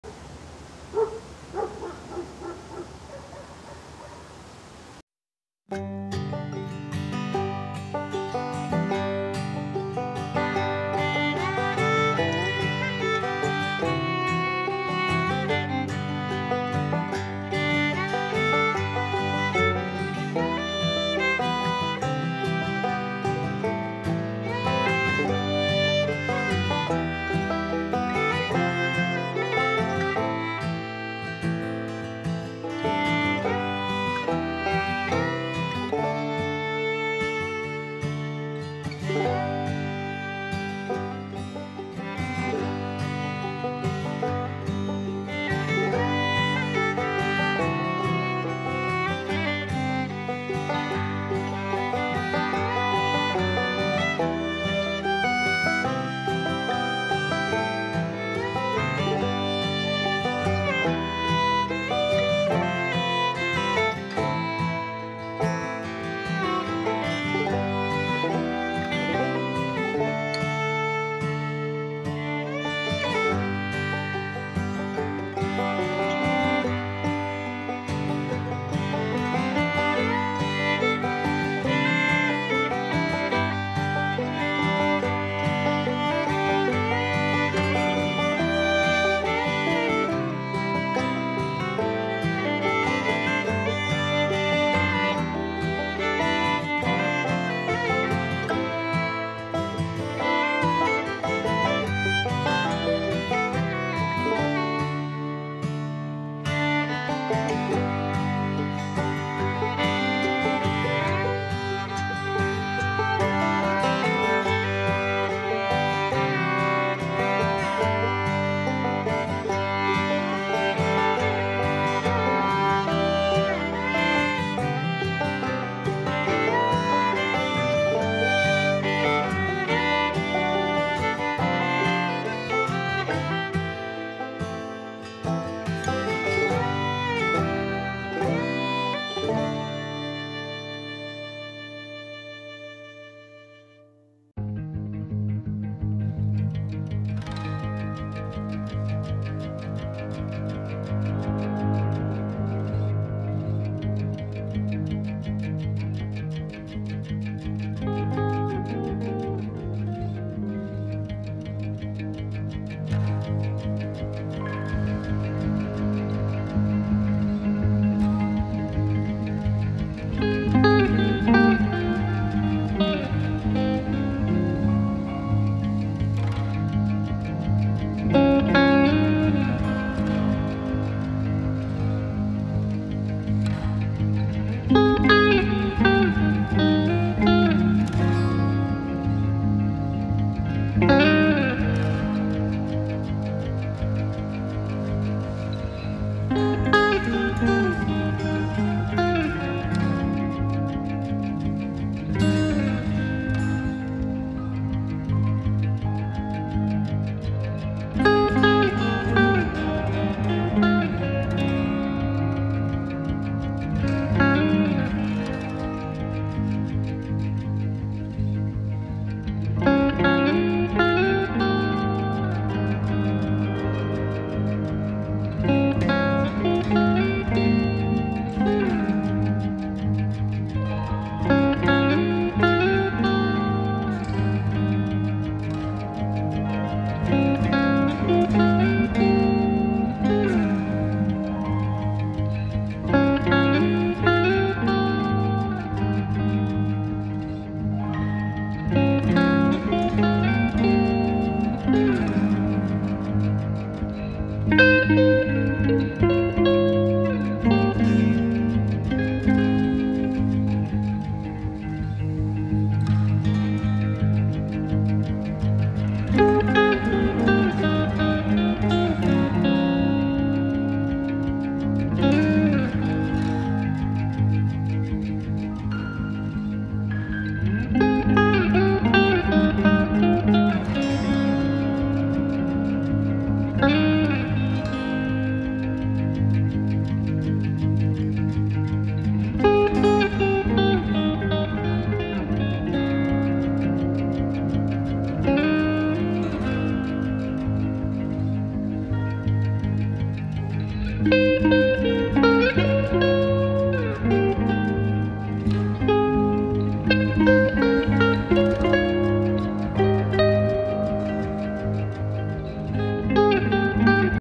Webster Summer 2021 Drone Footage
2021 flyover of the big town of Webster Iowa.